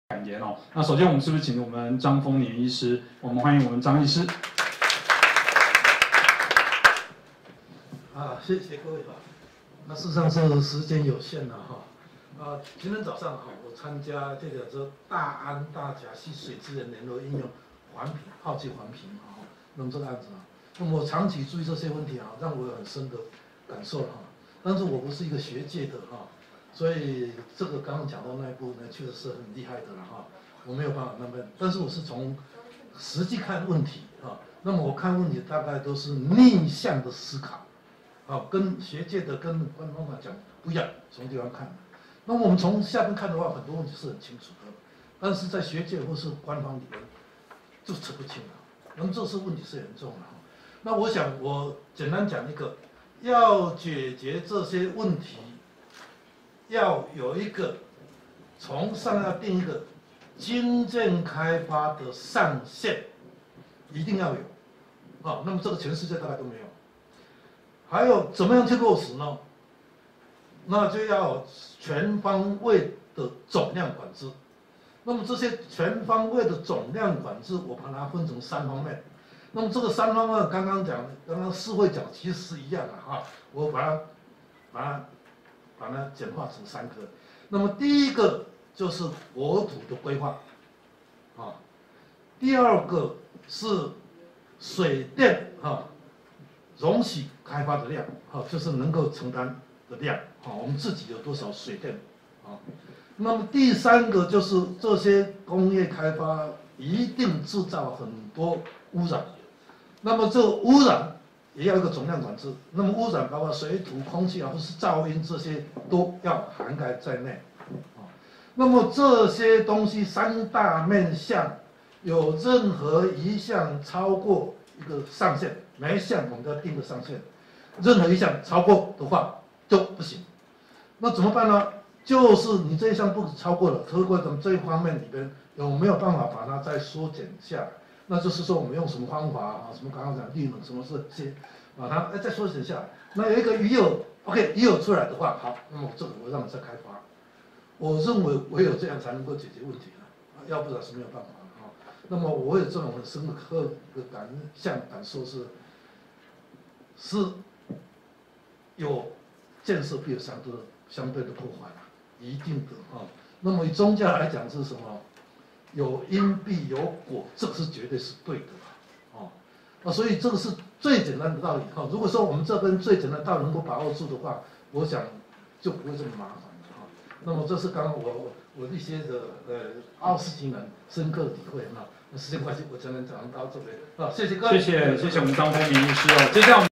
2020全國NGOs環境會議 環境保護終身成就獎得主 張豐年 醫師 致詞
2020全國NGOs環境會議 環境保護終身成就獎得主 張豐年 醫師 致詞

相關報導 :
鏡週刊

【為山河動刀1】為檢舉冷氣噪音抗爭十年　他試用新機器竟發現自己肝硬化